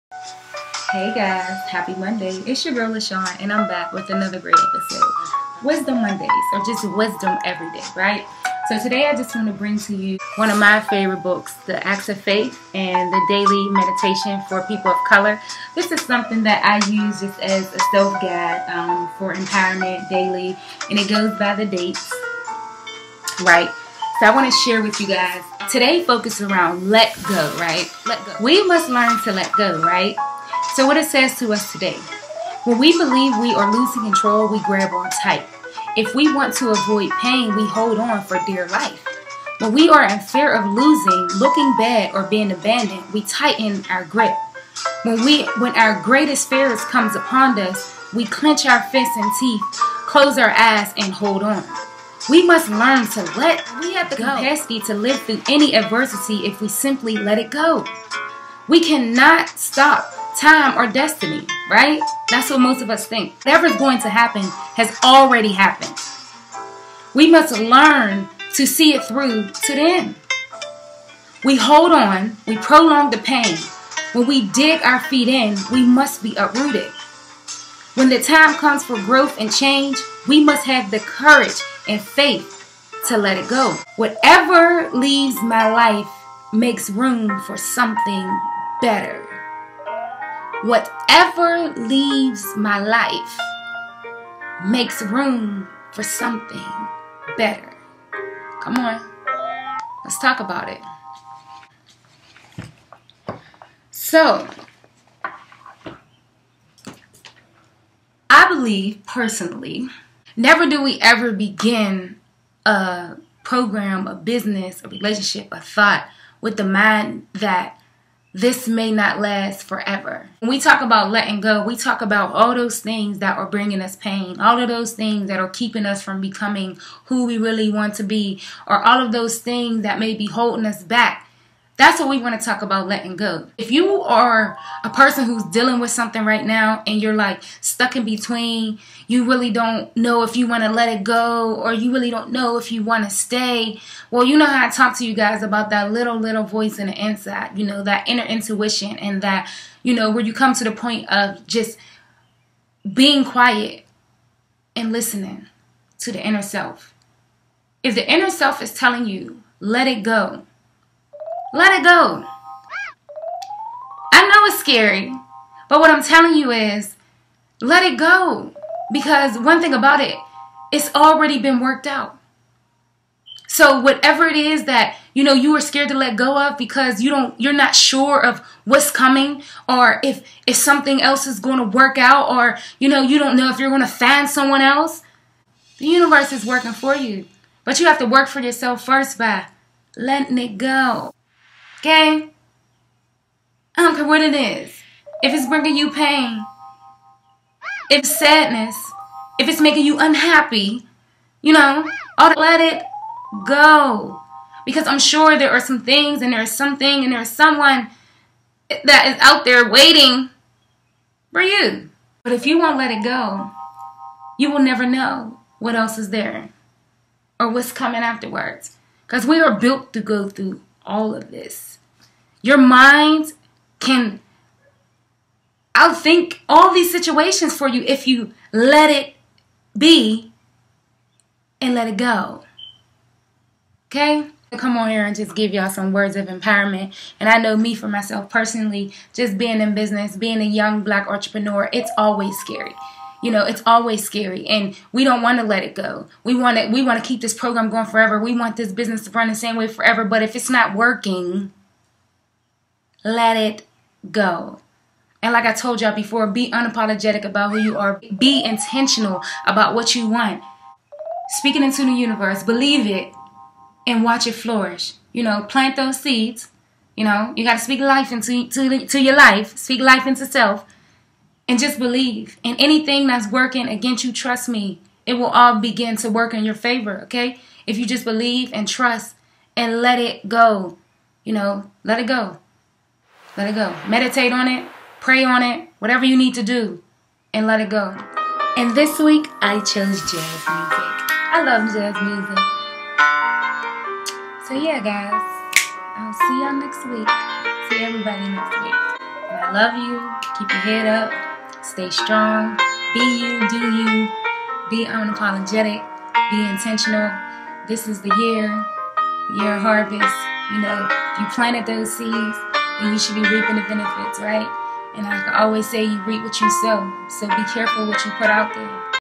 Learning how to “Let Go”!
In this video we will cover today’s  “ACT OF FAITH” Daily Meditations For Color People. By: IYANLA VANZAN ! Discussing the topic “Let It Go”